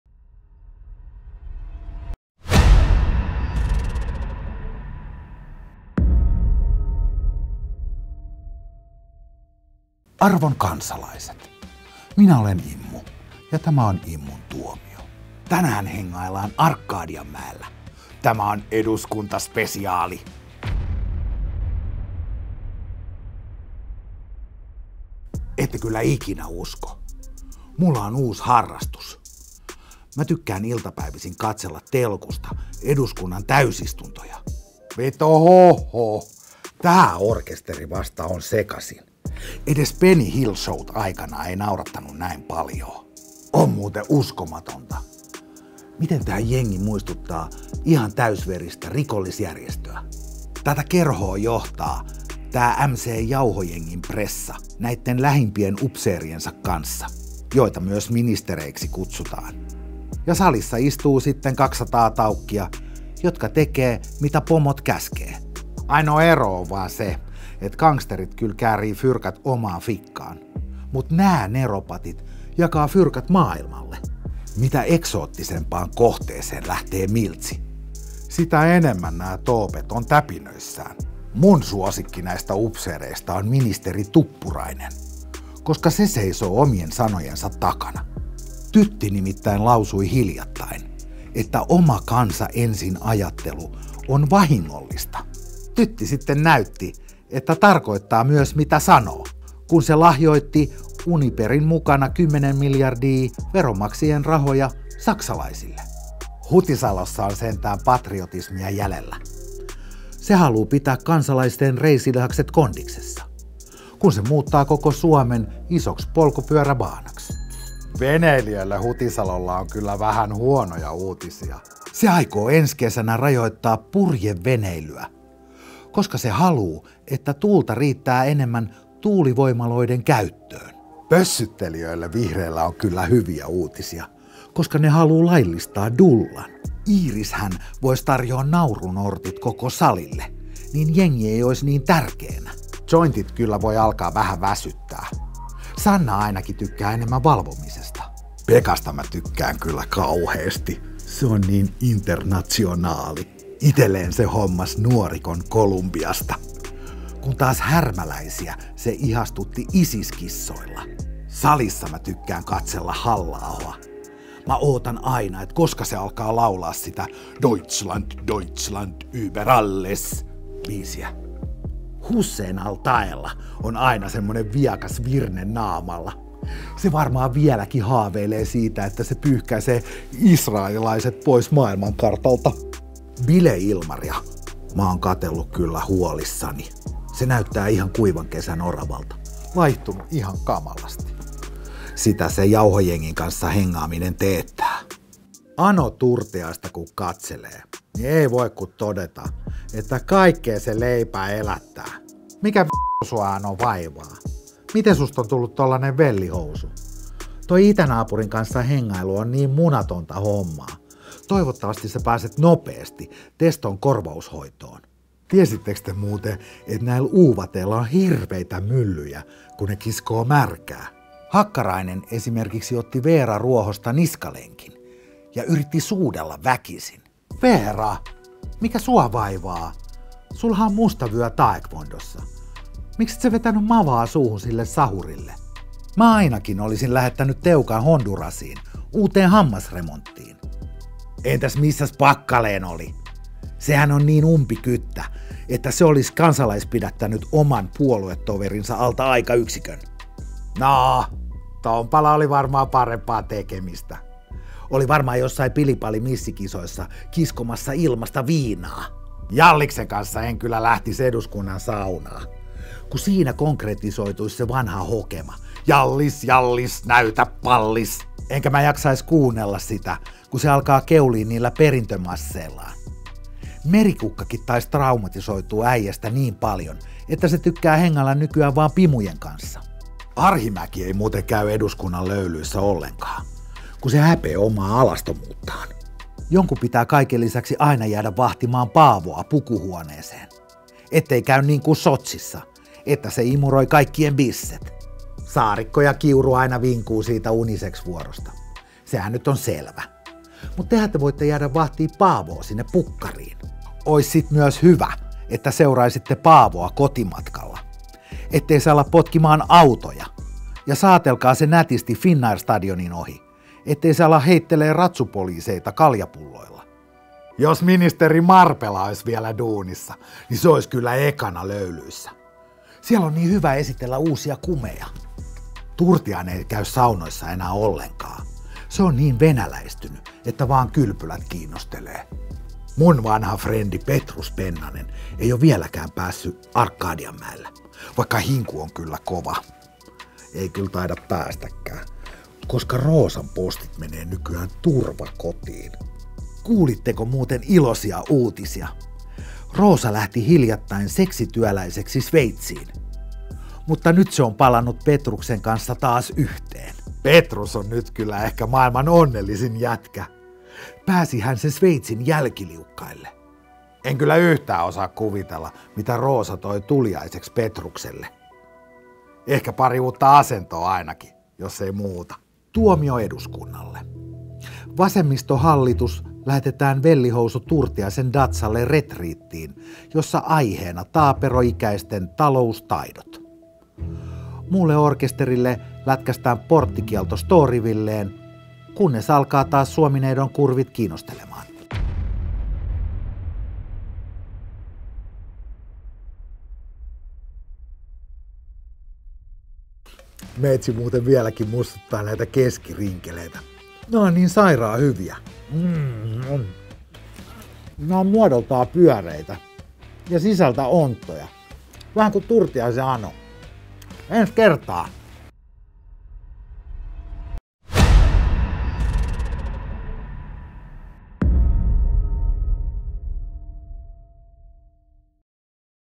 Immun tuomio. Eduskunta-spesiaali.
Tällä kertaa tuomionsa saa koko Arkadianmäki!
Totuttuun tapaan tuomioita ropisee mitään säästelemättä!